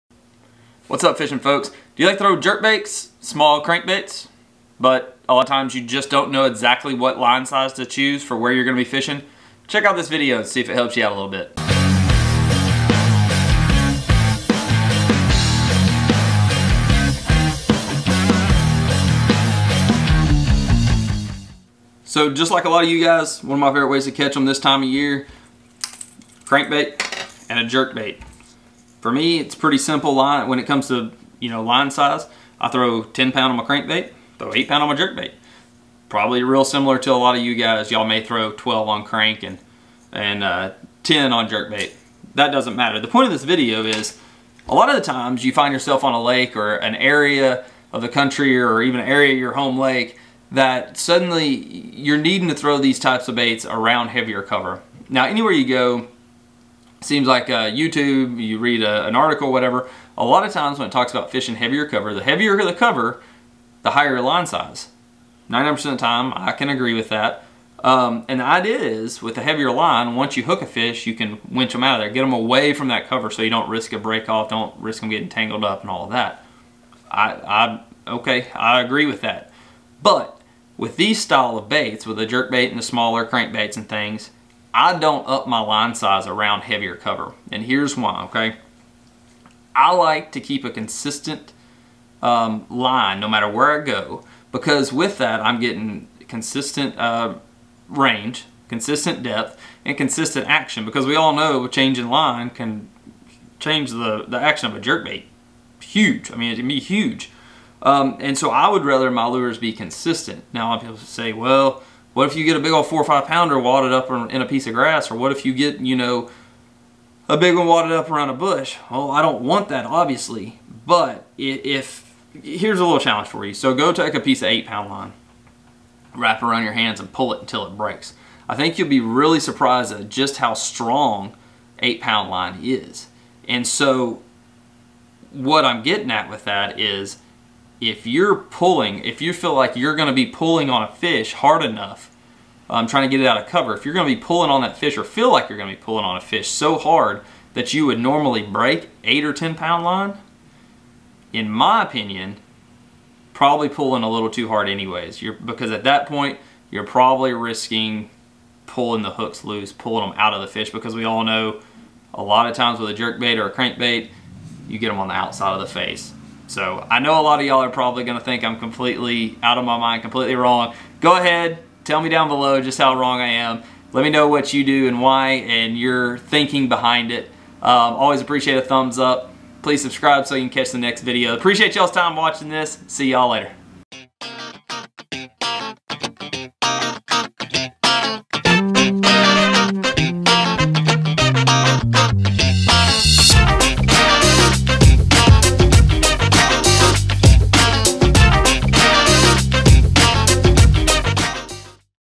LIGHT line for HEAVY cover
Normally we are told to use heavier line around heavier cover. Here is why I DON'T!